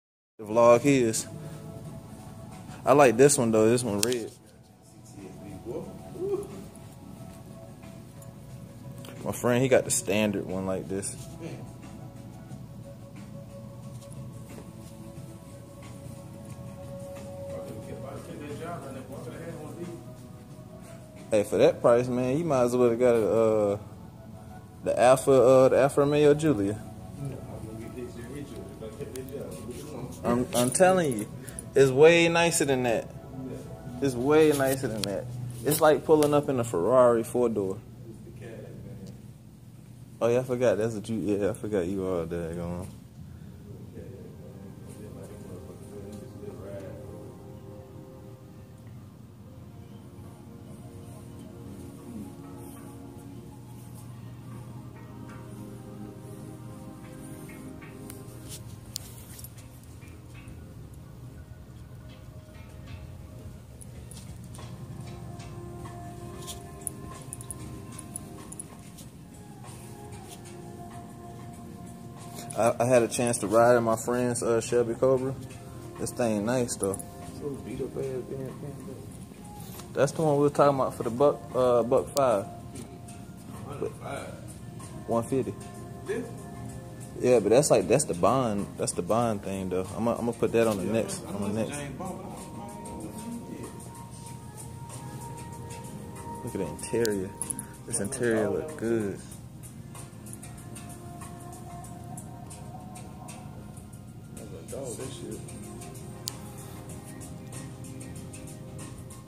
Very nice Shelby Cobra for sale at Taggart
Only a few words to describe this rare beautiful car. Superb is the word its amazing in all ways come buy this rare opportunity now at Taggart of Cary NC tell them Gio sent ya! Facebook.com/AnythingWithMotors. Youtube.com/AnythingWithMotors